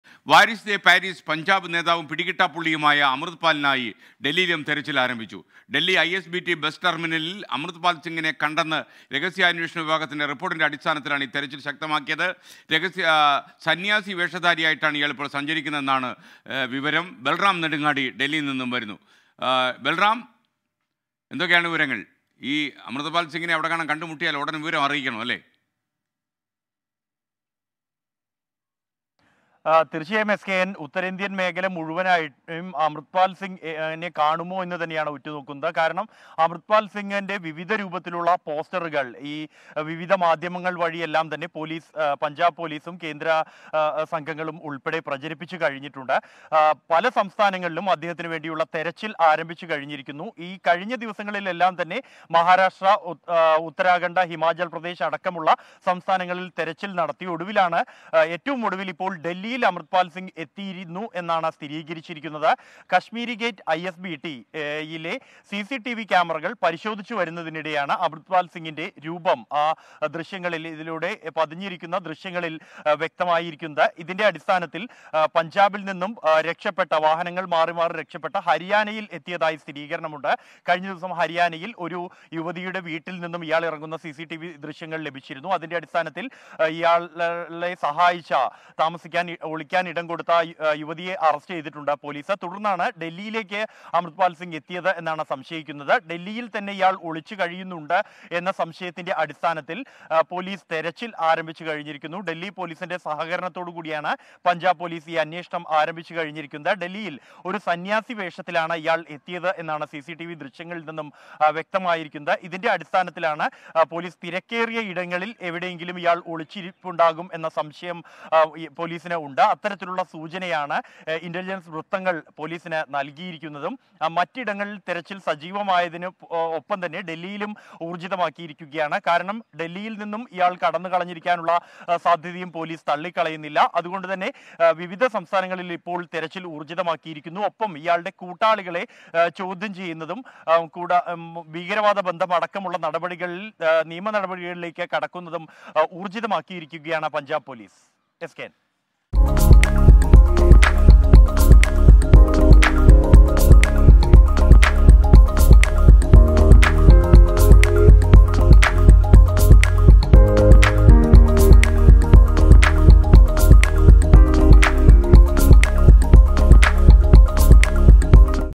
ബസ് ടെർമിനലിൽ അമൃത് പാൽ സിങ്ങ്; ഡൽഹിയിൽ കണ്ടെന്ന് രഹസ്യാന്വേഷണ വിഭാഗം;
അമൃത് പാലിൽ സിങ്ങിനെ ഡൽഹിയിൽ കണ്ടെന്ന് രഹസ്യാന്വേഷണ വിഭാഗം; സന്യാസവേഷധാരിയായി സഞ്ചരിക്കുന്നു എന്നാണ് വിവരം